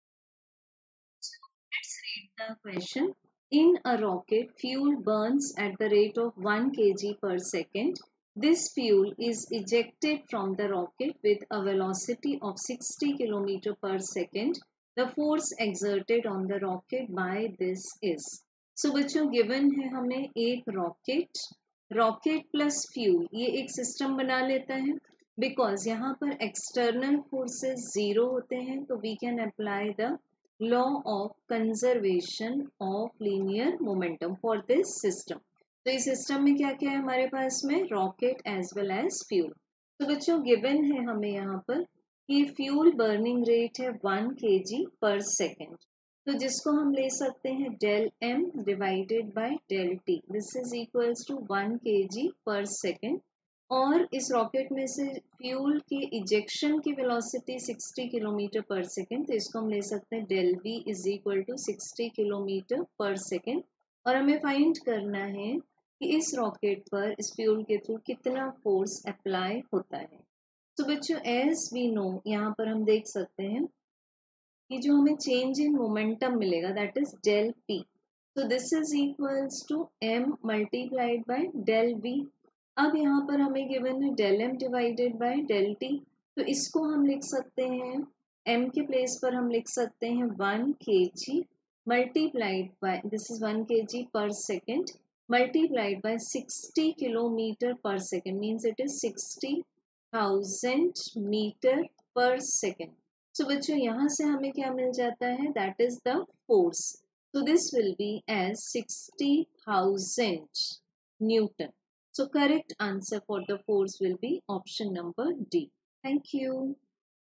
In a rocket, fuel burns at the rate of 1 kg/s. This fuel is ejected from the rocket with a veloc....
In a rocket, fuel burns at the rate of 1 kg/s. This fuel is ejected from the rocket with a velocity of 60 km/s. The force exerted on the rocket by this is 📲PW App